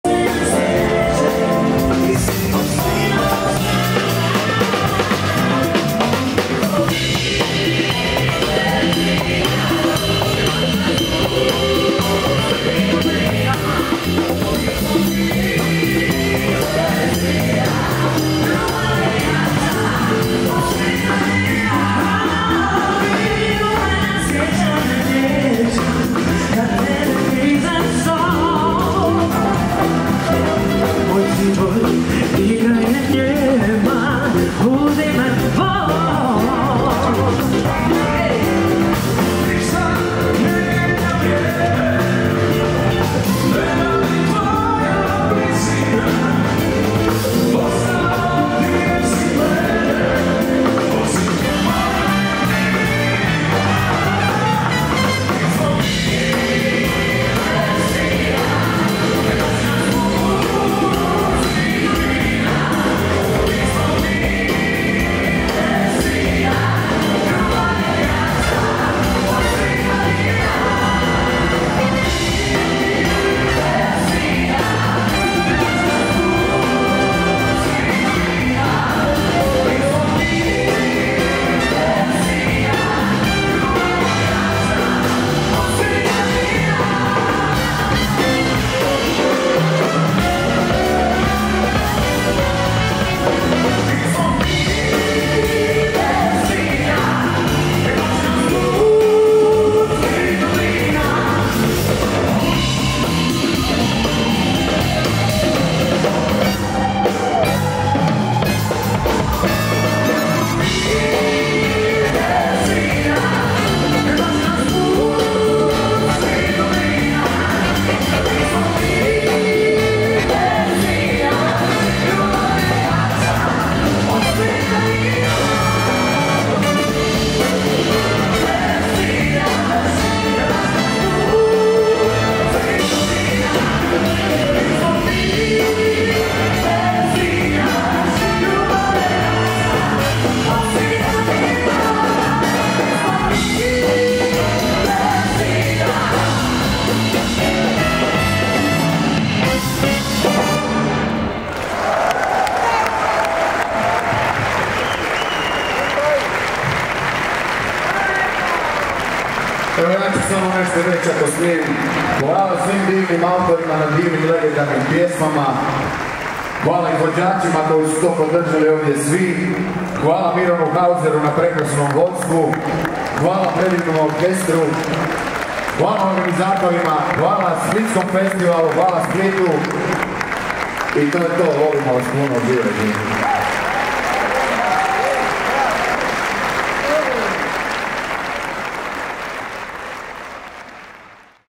Zadnja izvedba 4. dan • Splitski Festival UHD • 4k • 2023
Zadnji nastup 4. dana na @splitski.festival, odličan performans svih pjevača i na kraju prekrasna izvedba ove pjesme kao finale večeri 😊❤️📸☺️

Korištenje videomaterijala je strogo zabranjeno bez upita!